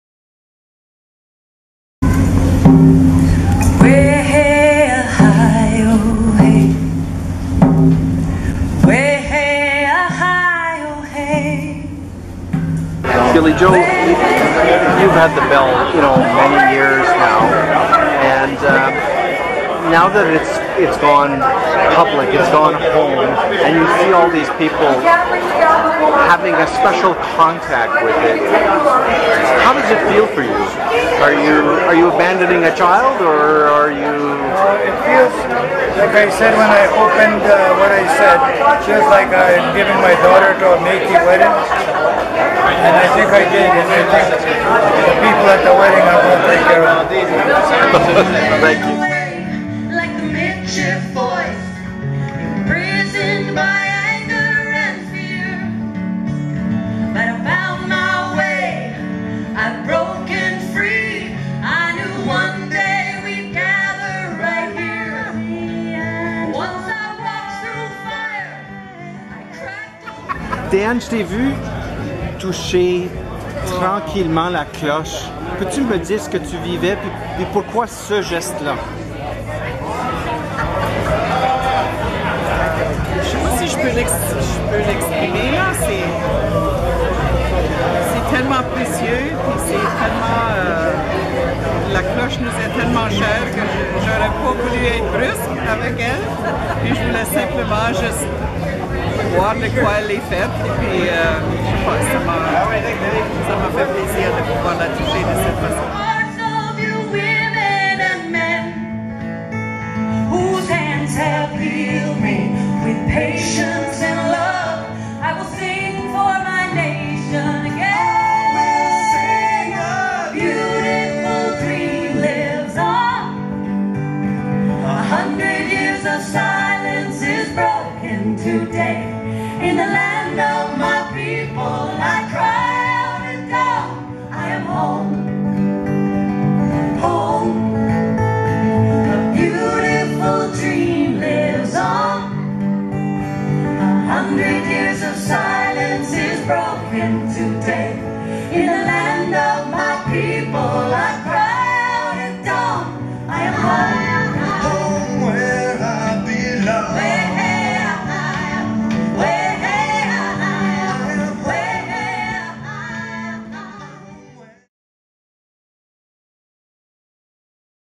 La cloche de Batoche - Témoignages et réactions
« Marie-Antoinette » ayant été dévoilée à Batoche, les Métis ont eu l'occasion de la contempler de près. Dans cette vidéo, plusieurs Métis réagissent à l'expérience. De plus, Billyjo DeLaRonde, longtemps détenteur de la cloche, explique comment la célébration du 20 juillet dernier ressemblait à une noce où il aurait donné une fille en mariage.